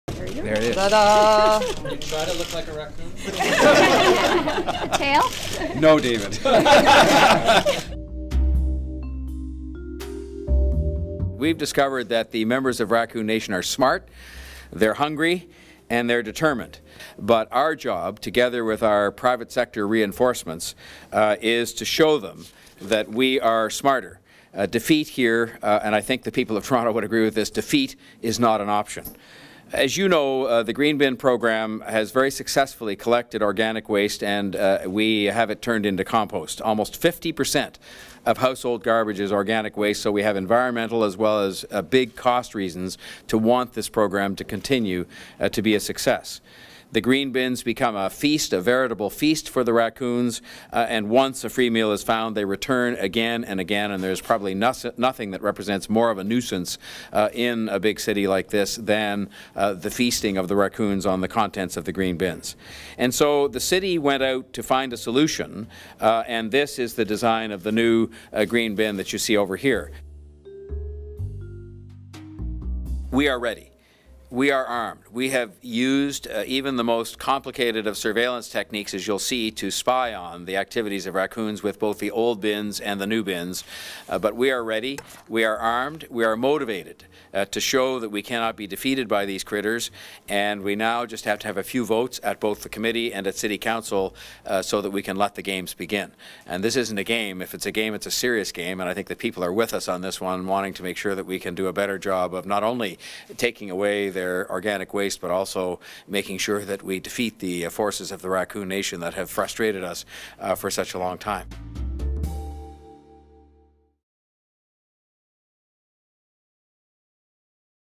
Toronto Mayor John Tory vs. 'Raccoon Nation'
Toronto Mayor John Tory unveiled new green bins designed to prevents the city's raccoons from accessing the content